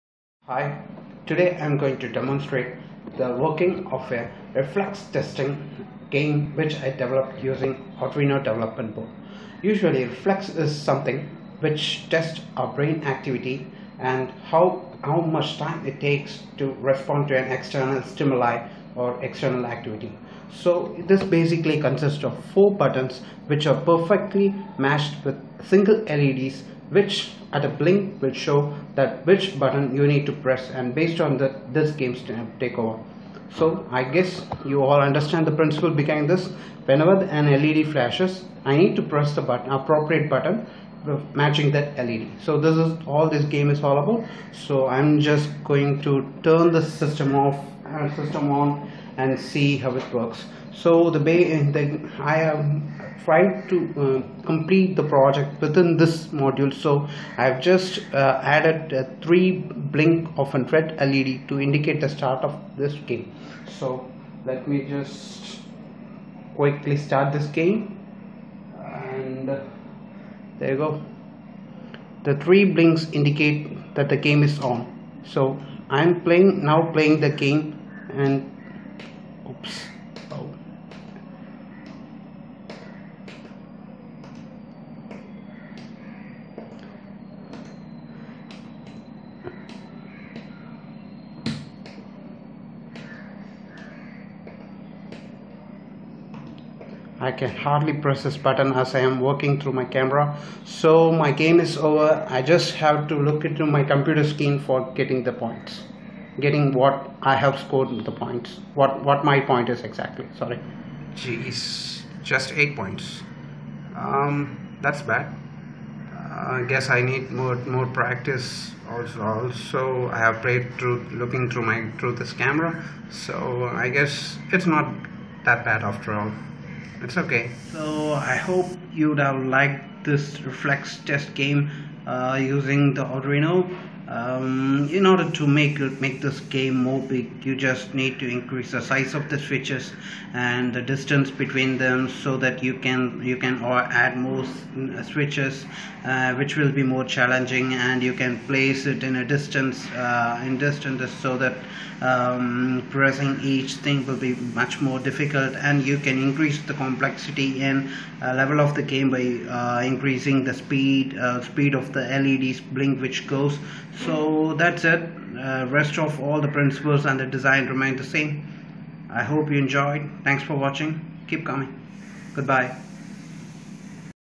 Reflex test Game using Arduino
Make your own Reflex test / trainer game using Arduino uno Development board .